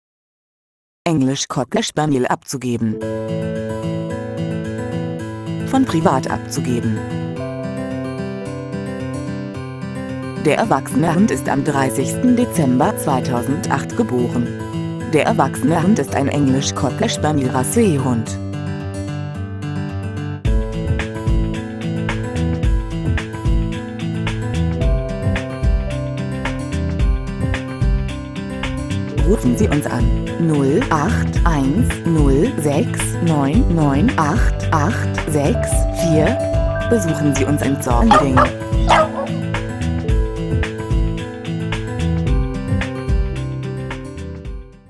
Englisch Cocker Spaniel in Zorneding
Leider müssen wir unseren 2-jährigen unkastrierten Cocker Spaniel abgeben. Er ist gegenüber fremden Kindern etwas aggressiv, ansonsten aber ein absoluter Schmuser., Er ist total verspielt und ein ...